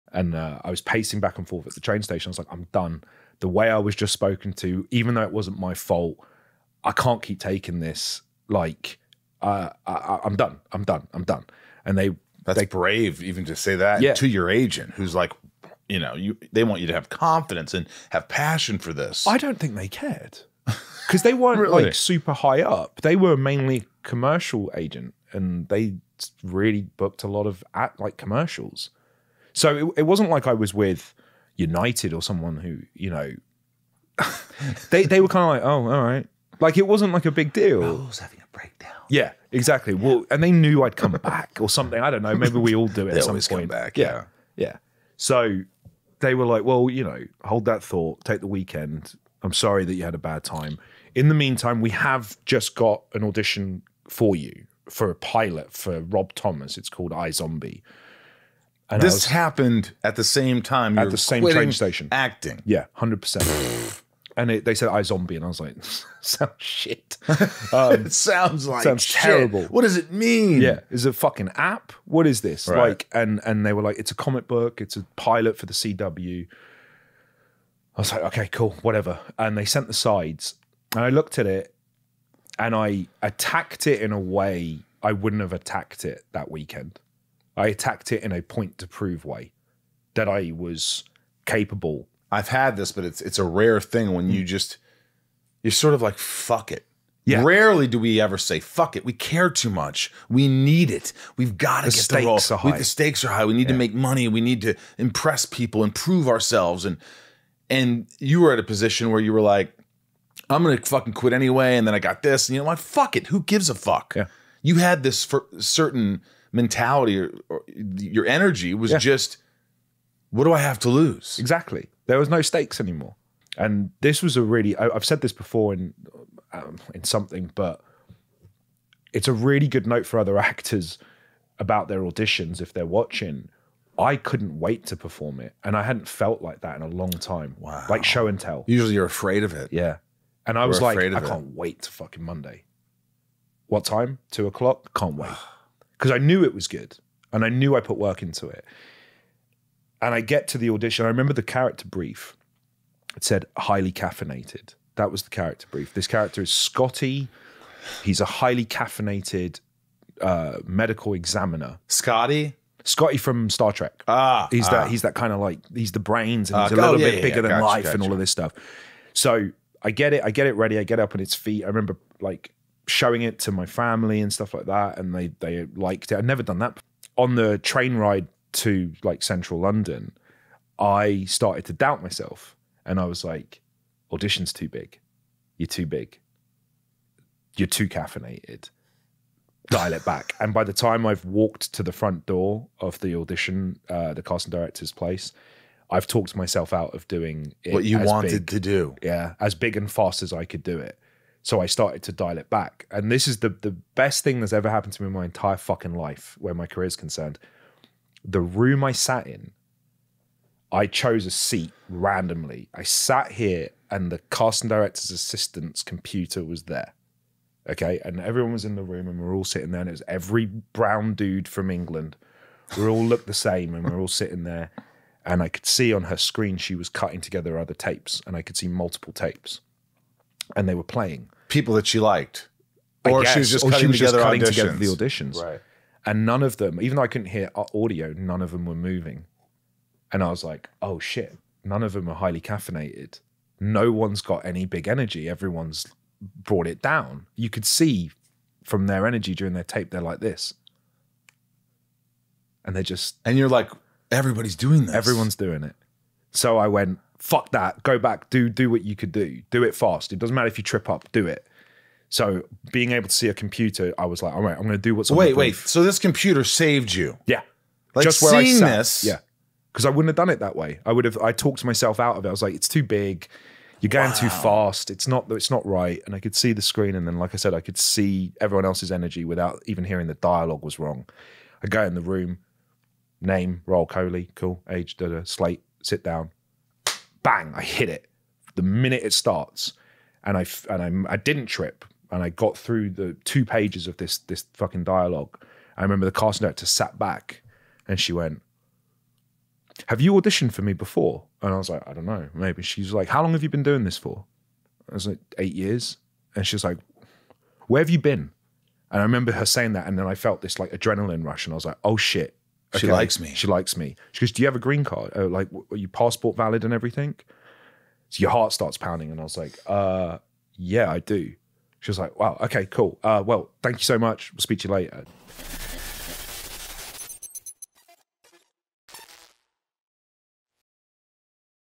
How his audition for iZombie saved Rahul Kohli from quitting his career as an actor
Sometimes when you're at you're lowest, all it takes is a little twist of good fate to put you right back on your feet...

Rahul Kohli (iZombie, Midnight Mass) joins us this week to tell his inspiring story of how a lucky twist of fate, while he was on the brink of quitting acting because of a horrible audition, landed him his life changing role in iZombie. Rahul explains, however. that the onset politics, stress of being on a bubble show, and shooting in Canada caused him to spiral into unhealthy abusive and unhealthy habits. We also talk about his deep relationship personally and professionally with Mike Flanagan, the typecasting of South Asian actors, and his experience being diagnosed with bipolar.